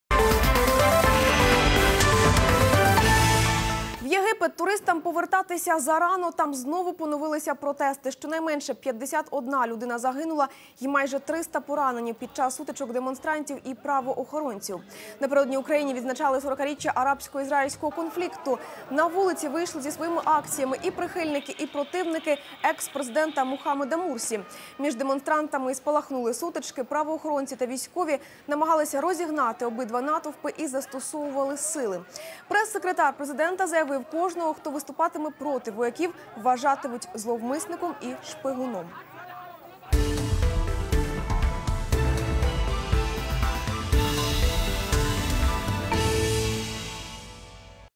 В Египте во время праздника убили 51 человека
Накануне в стране отмечали 40-летие арабо-израильского конфликта. На улицы вышли со своими акциями сторонники и противники экс-президента Мухаммеда Мурси

Напередодні в країні відзначали 40-річчя арабсько-ізраїльського конфлікту. На вулиці вийшли зі своїми акціями прихильники і противники екс-президента Мухамеда Мурсі